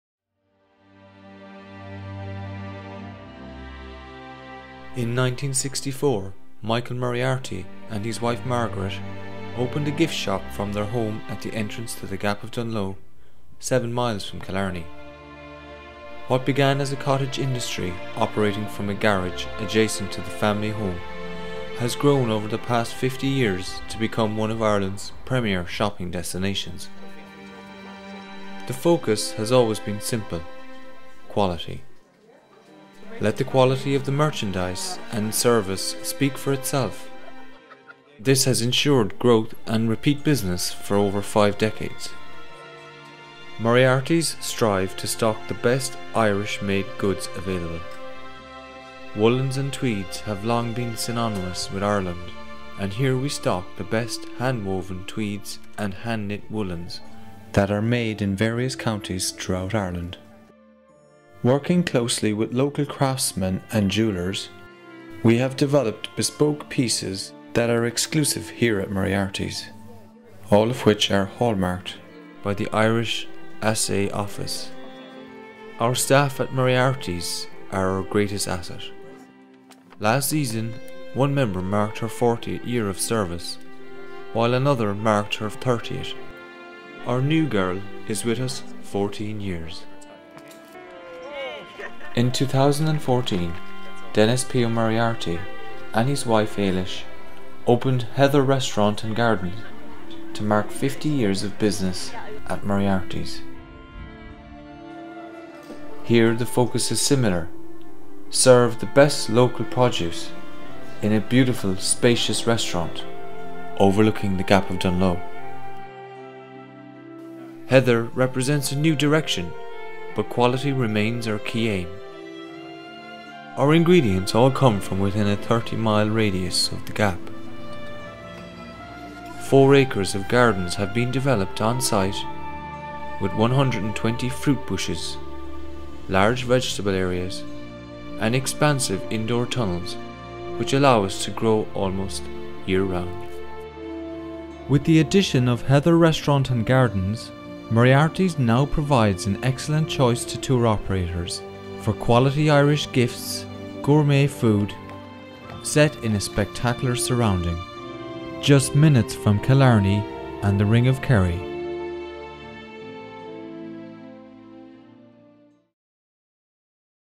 A ViSIT TO MORIARTY'S AND HEATHER RESTAURANT AT THE GAP OF DUNLOE
IrelandRnR.com Tours visits Moriarty's for luxurious Irish wool goods and beautiful finely crafted Irish jewelry.  Our tour guests dine in the spacious Heather restaurant overlooking the Gap of Dunloe.  Heather serves delicious organic Irish lunches and dinners prepared by Heather's chefs from local meats, seafood, and produce.  Take a look at  IrelandRnR.com for you choice of Ireland reunion and recreation tours.